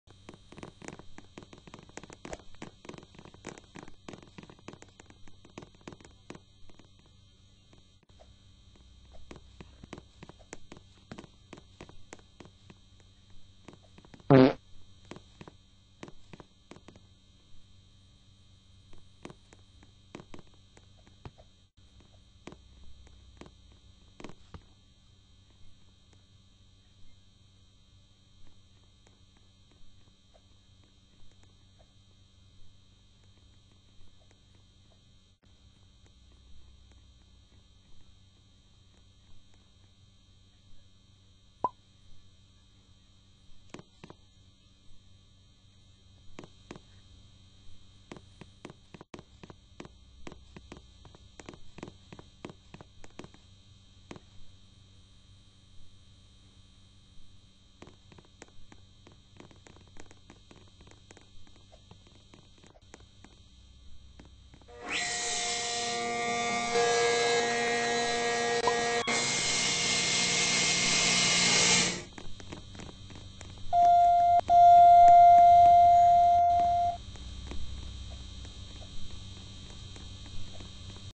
schindler2232 vs penguinlift12
this was at mrgoodbeans place but penguin and schindler meet face to face and things do not go down well who do you want to support a schindlerfan2233 or penguinlift12